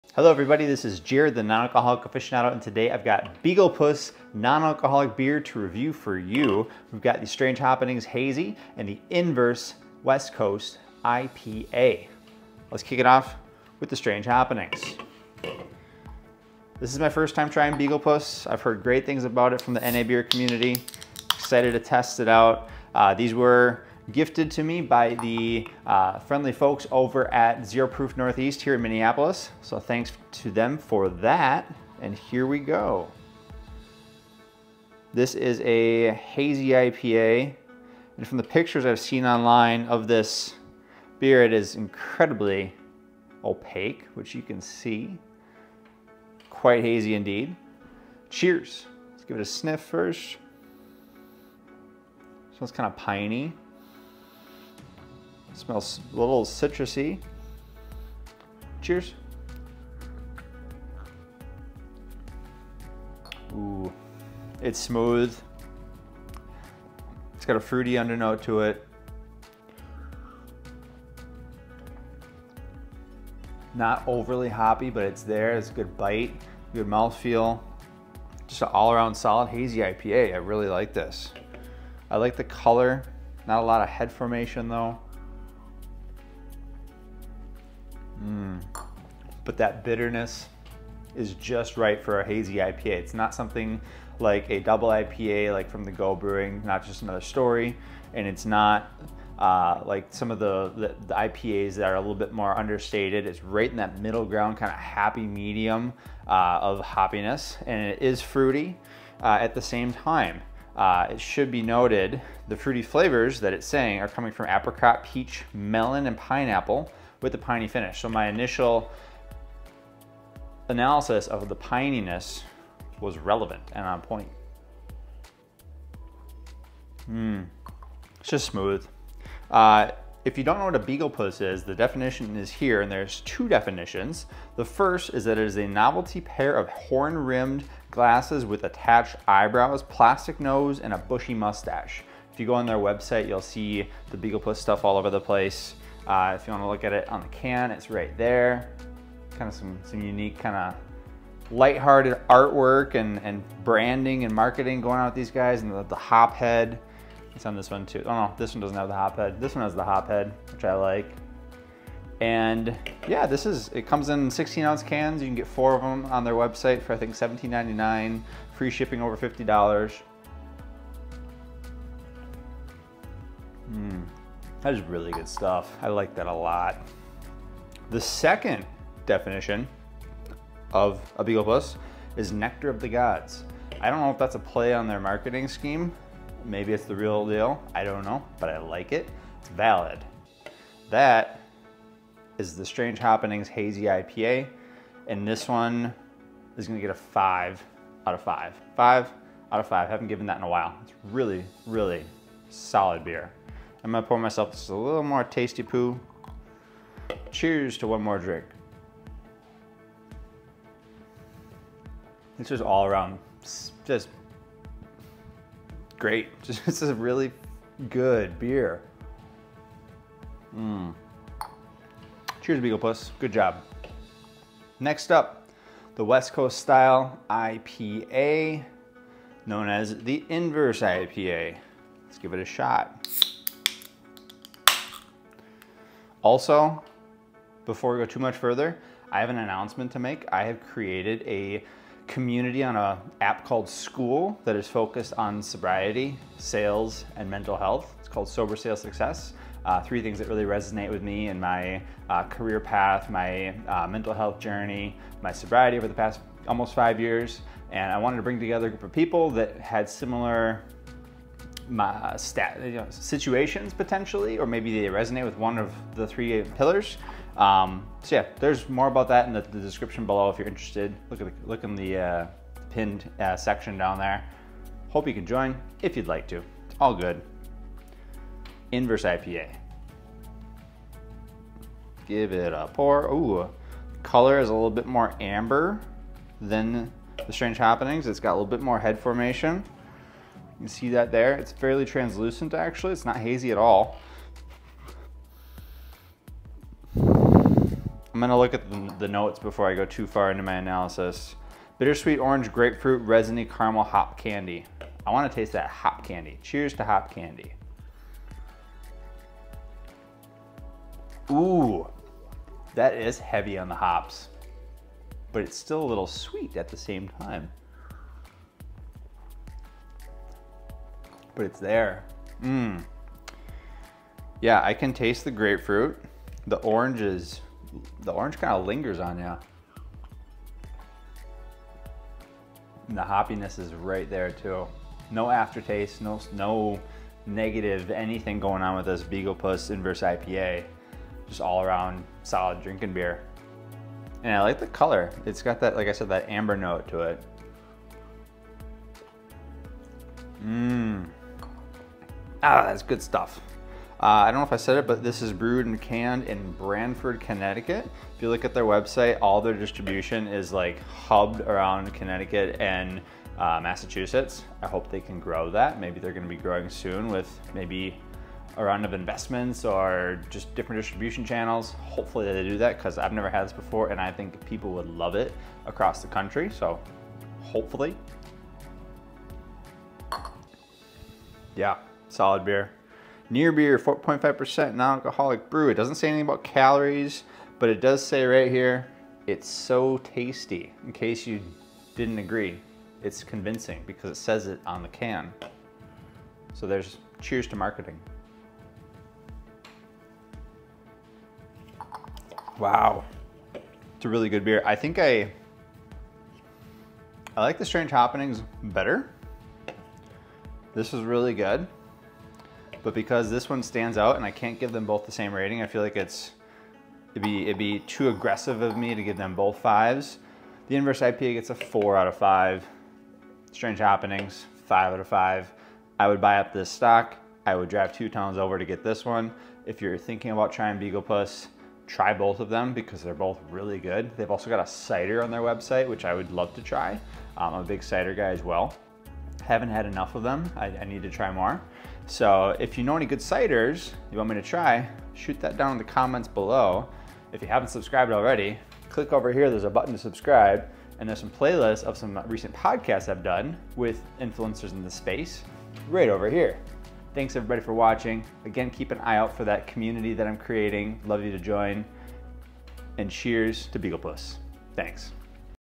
Beaglepuss Non Alcoholic IPA Review: Strange Hoppenings & Inverse West Coast IPA
In this taste test, we review two Non Alcoholic IPAs from Beaglepuss Brewing. First is the Strange Hoppenings Hazy IPA, which has big hoppy flavors that rival traditional IPAs. Next, we try the Inverse West Coast IPA, which is packed with bold flavors and a perfect balance of hops. If you're exploring the world of Non Alcoholic craft beers, this video will look at some unique and tasty options from Beaglepuss!

**BEAGLEPUSS BEER RATINGS**
✅ Strange Hoppenings Hazy IPA: 5 outta 5 (Would buy up the stock)
✅ Inverse West Coast IPA: 4 outta 5 (Would drive 2 towns over to get it)

☑️ Timestamps
00:00 Intro
00:12 Strange Hoppenings Hazy IPA
03:45 Inverse West Coast IPA
07:09 Wrap-Up

This video is about Beaglepuss Non Alcoholic IPA Review: Strange Hoppenings & Inverse West Coast IPA. But It also covers the following topics:

Inverse West Coast IPA
Best Non Alcoholic IPAs
Non Alcoholic Craft Beer